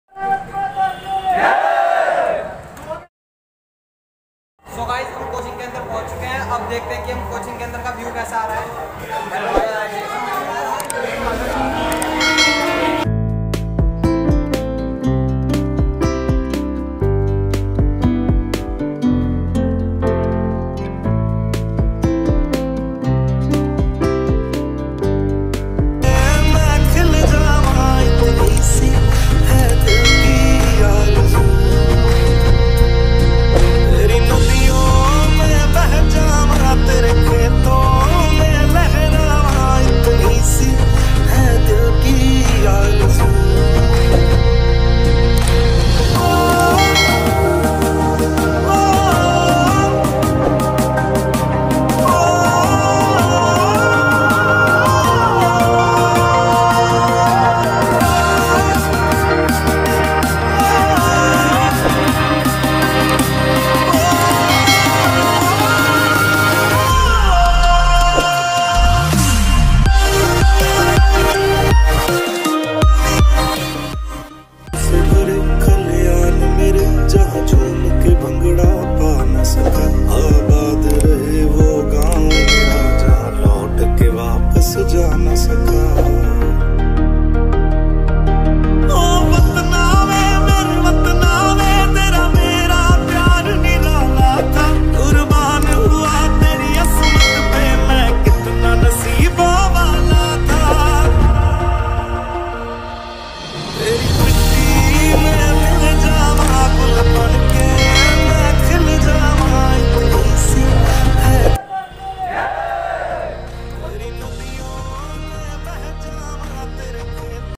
Independence day 🇮🇳 special vlog 😘 by Vintage Vlogs | Vipin Raj Rana
Independence Day Special vlog 🇮🇳||

hey guys welcome to my youtube channel to guys Aaj ka vlog very interesting hone wala hai kyoki aaj ke is vlog me mai aaplogo ko independence day special vlog dekhane wala hu .

aur aaj ke is vlog me hamlog bhi is mahotsav ka hissa honge to please aaplog video ko last Tak dekhiyega.